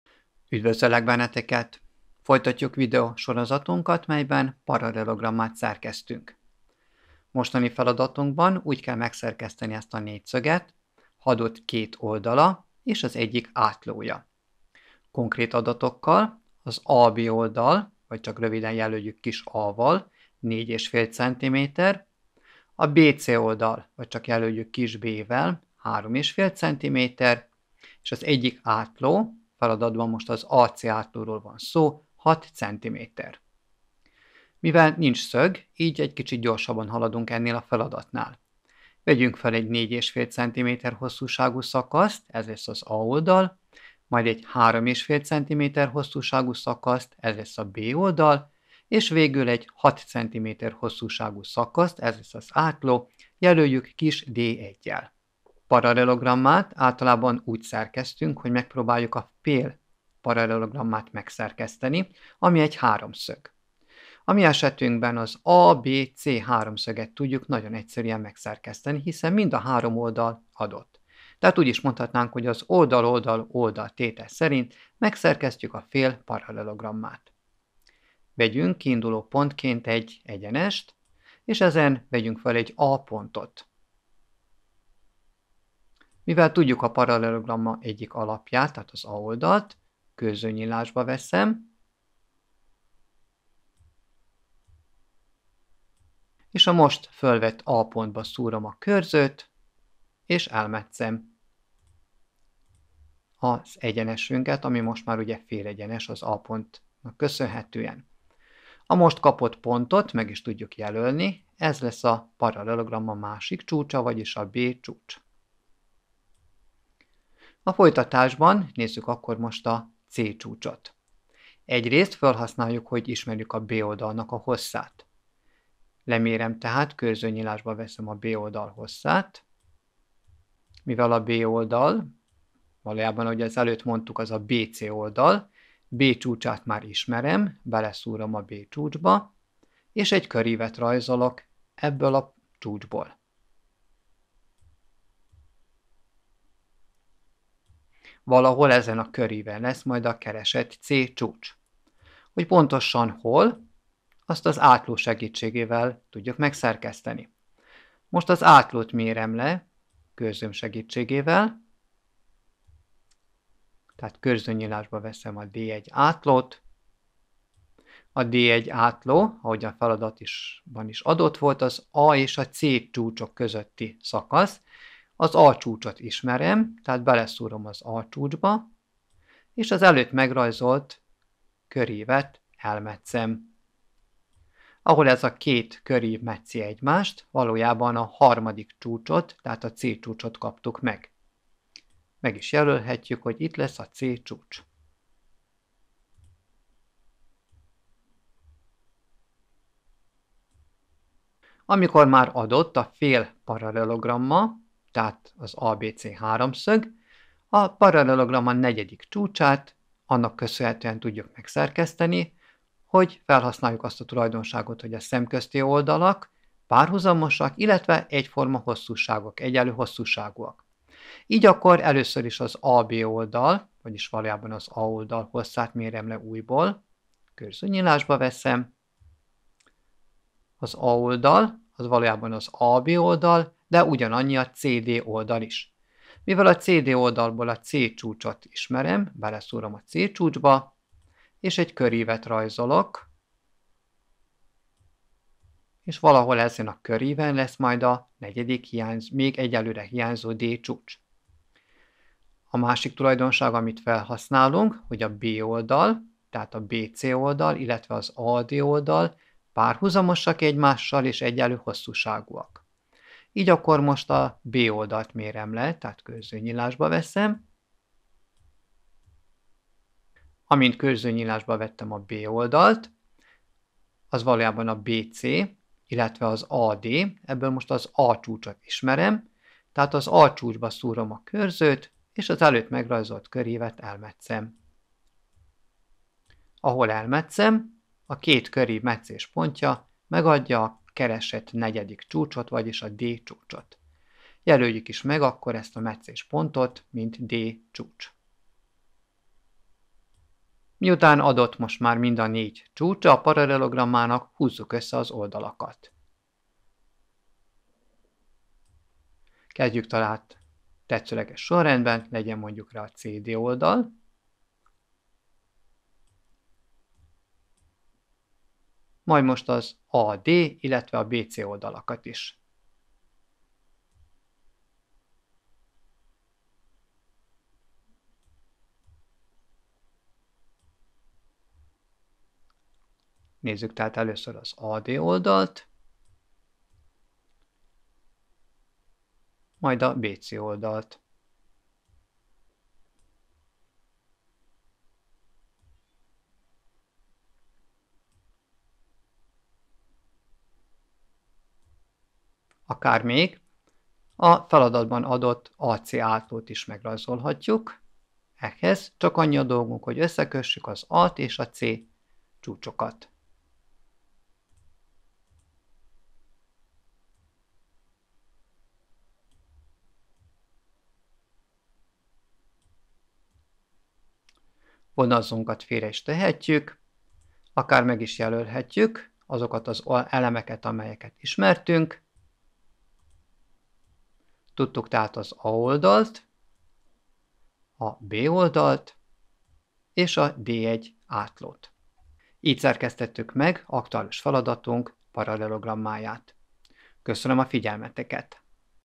2) PARALELOGRAMMA SZERKESZTÉSE – ISMERT KÉT OLDALA ÉS AZ EGYIK ÁTLÓJA (a=4,5 cm, b=3,5 cm, d1=6 cm)
Ebben a leckében a paralelogramma szerkesztését gyakoroljuk.

A teljes sorozat megtalálható az alábbi linken
PARALELOGRAMMA SZERKESZTÉSE